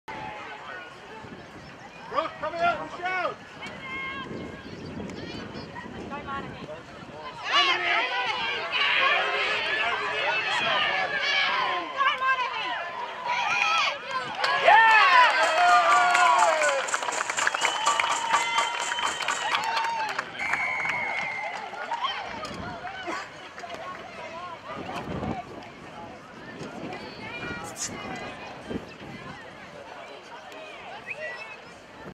2016 William Ward Try ~ CDJRL Under 12 Grand Final ~ Kangaroos v Central Scorpions
William Ward scores in the corner for Kangaroos in the 2016 Under 12 Grand Final at Jones Park on 27th August.